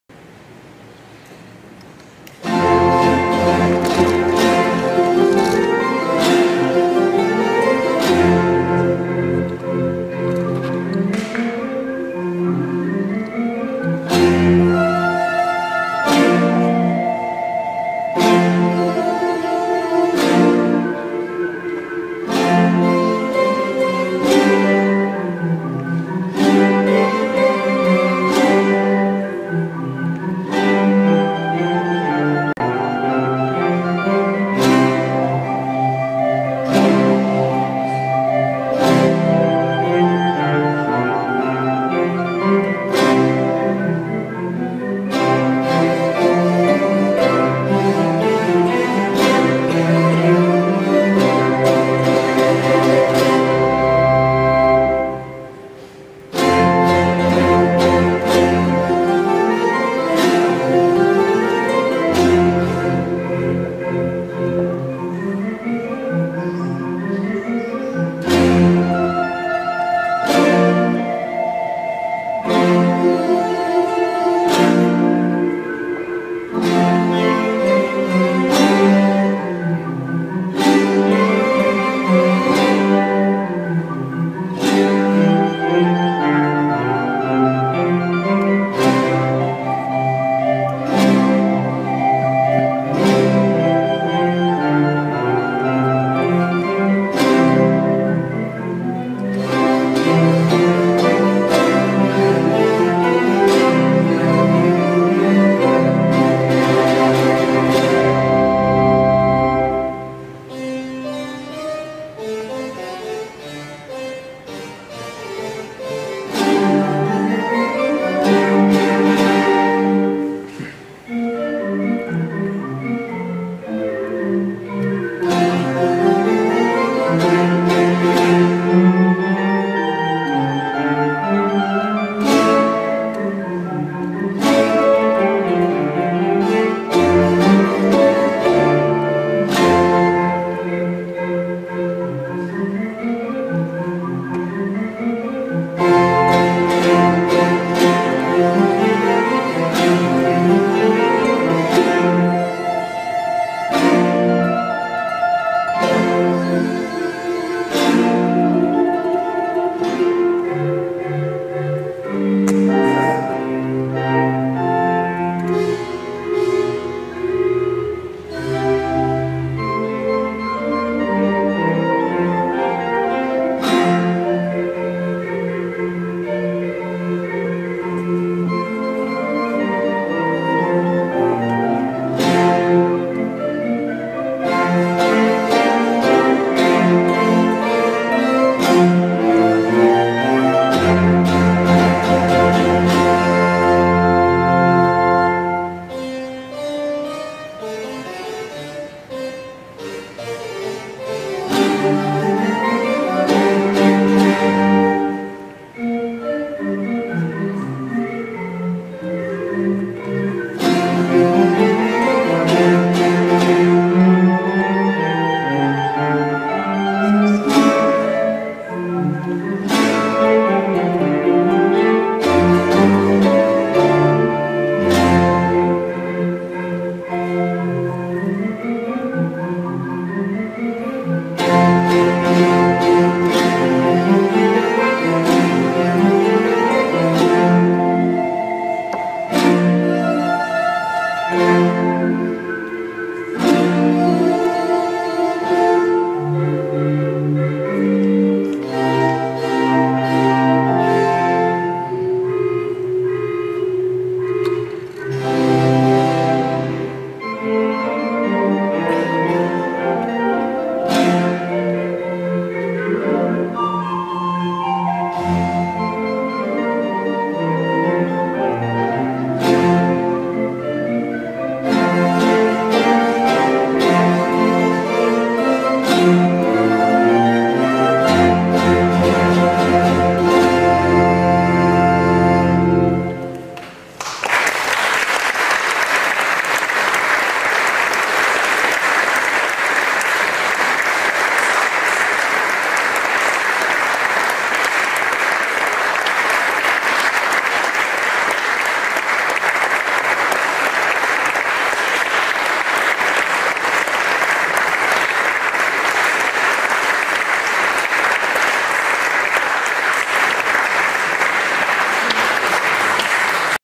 ENSEMBLE SUPER FLUMINA BABYLONIS_POGGIO TERZA ARMATA_17 8 2018 (2335 E 2335-1)
ENSEMBLE SUPER FLUMINA BABYLONIS nel concerto inaugurale a Poggio Terza Armata il 17 agosto 2018